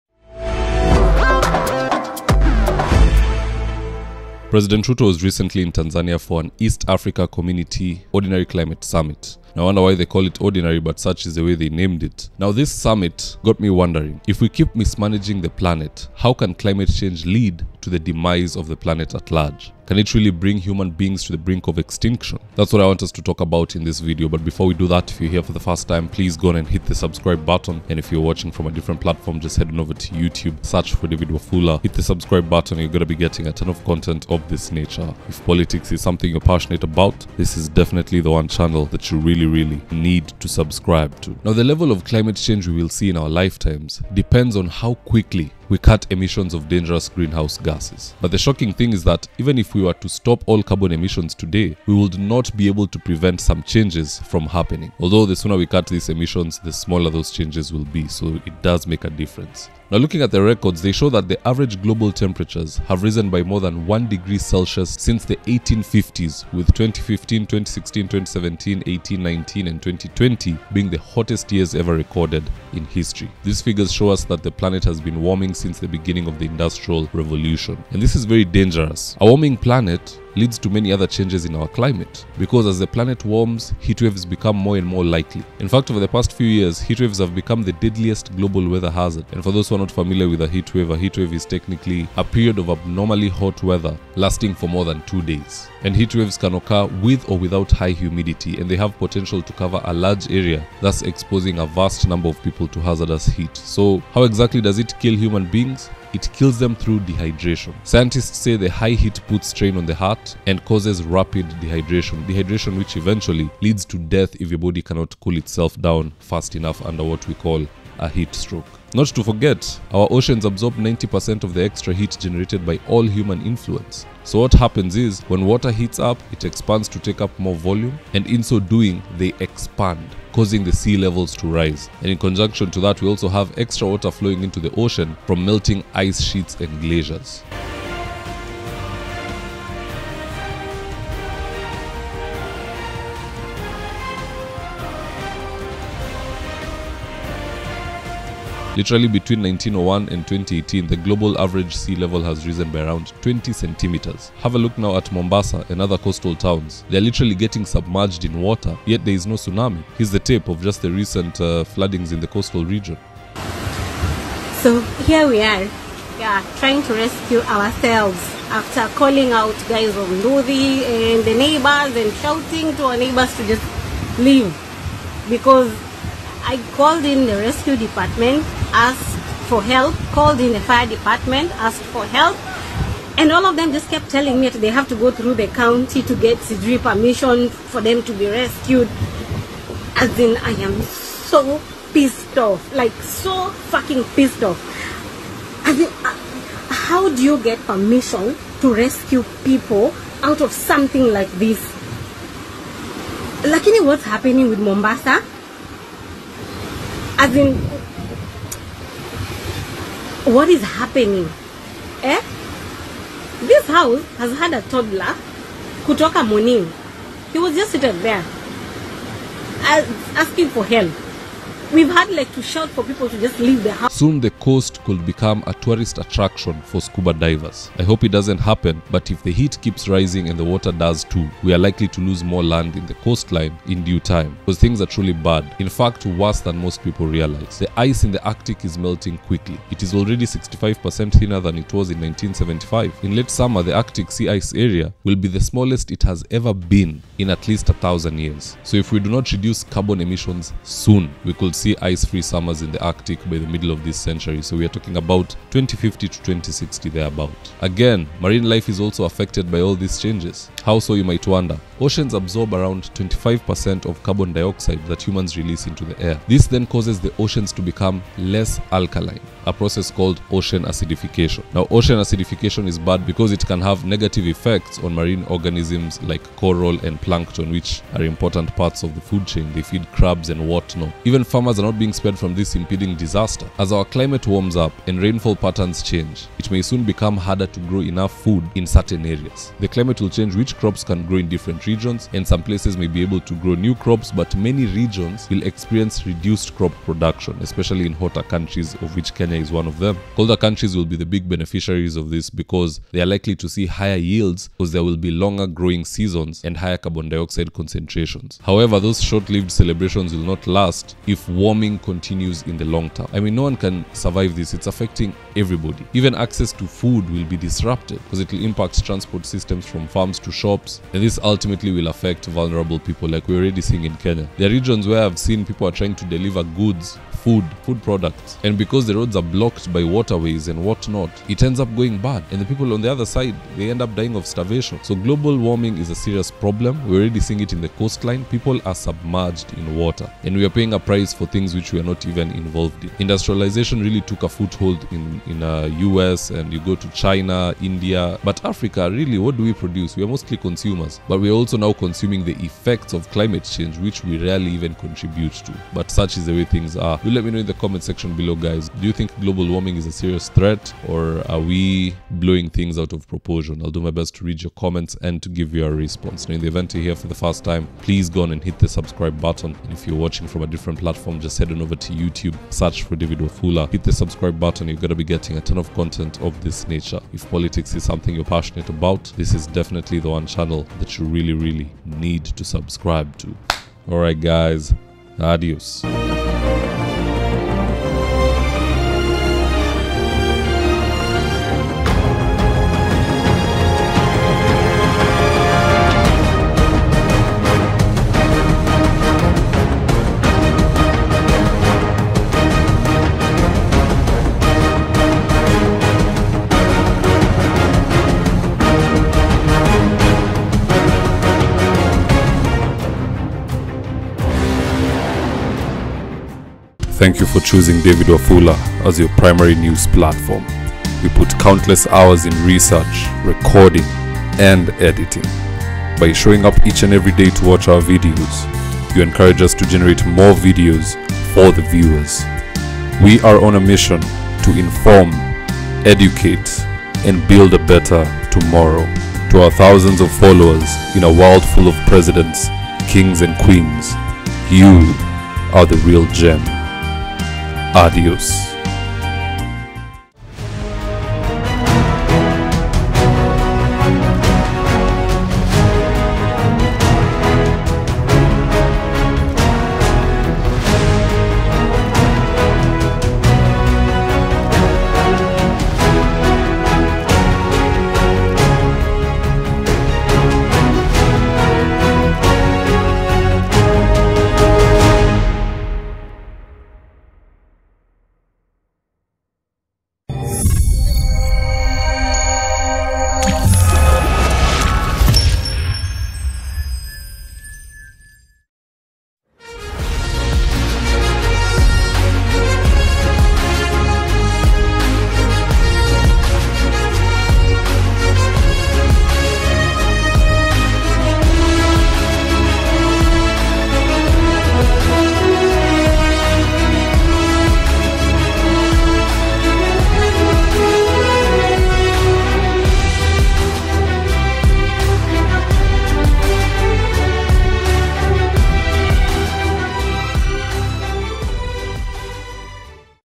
SHOCKING! HOW KENYA COULD LOSE THE ENTIRE TERRITORY OF MOMBASA AND KILIFI!
I am David Wafula, a political analyst and consultant.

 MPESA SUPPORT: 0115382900